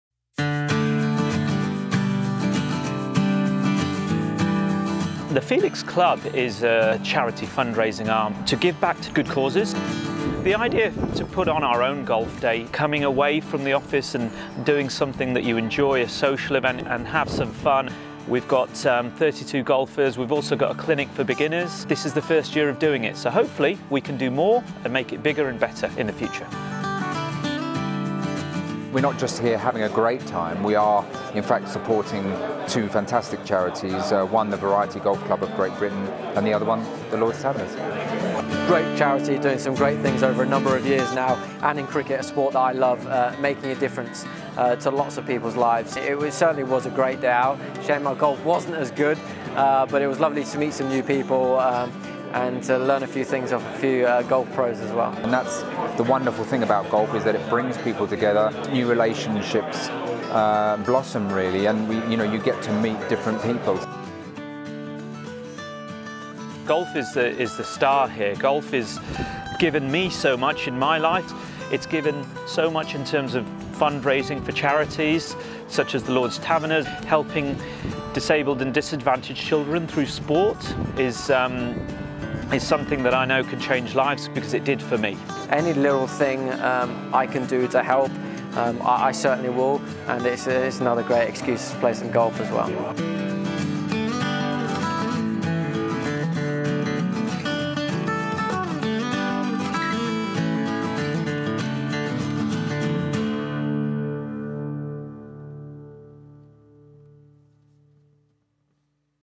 Felix Cup 2019
Watch 🎥 Earlier this month, the first felixcup golf event took place at Burhill Golf Club to help raise money for the Variety Club and Lord's Taverners with appearances from Mark Ramprakash, James Taylor and Sarah Stirk. A huge thank you to TheFelixClub and Crystal Sports Travel for the money you raised at this fantastic event! 🏌️‍♂️

Here is an overview of the day! 👀